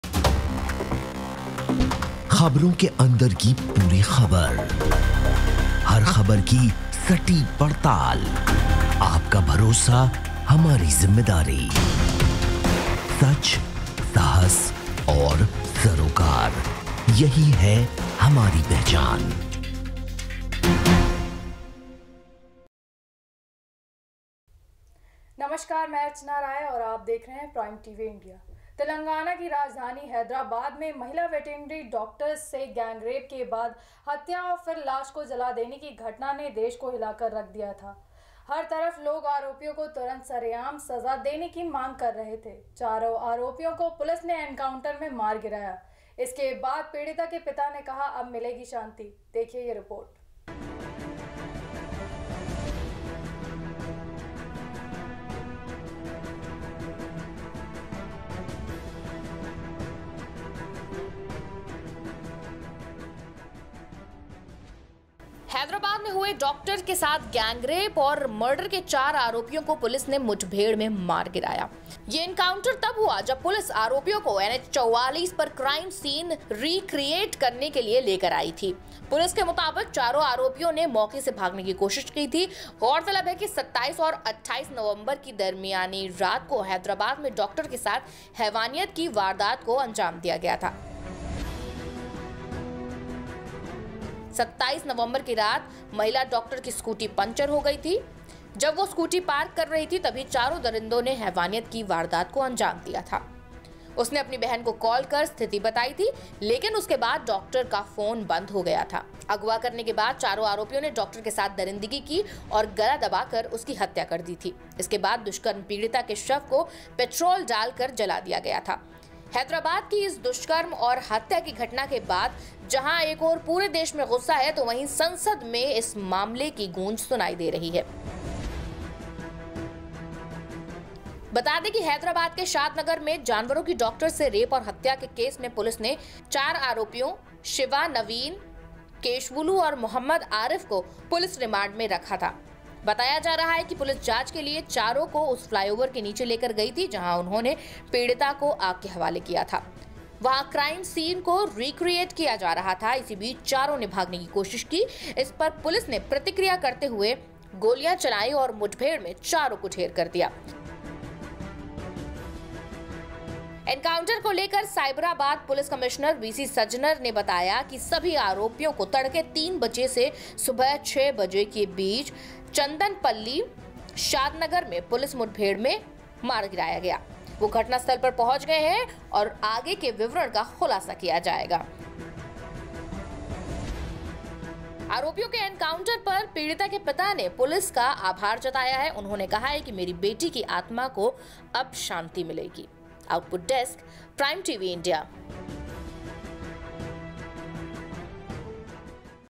हैदराबाद रेप के सभी चारों आरोपियों का एनकाउंटर..... पुलिस से हथियार छीनकर भाग रहा था एक आरोपी
तेलंगाना की राजधानी हैदराबाद में महिला वेटनरी डॉक्टर से गैंगरेप के बाद हत्या और फिर लाश को जला देने की घटना ने देश को हिला कर रख दिया था…. हर तरफ लोग आरोपियों को तुरंत सरेआम सज़ा देने की मांग कर रहे थे…..चारों आरोपियों को पुलिस ने एनकाउंटर में मार गिराया….इस के बाद पीड़िता के पिता ने कहा- अब मिलेगी शांति…  ।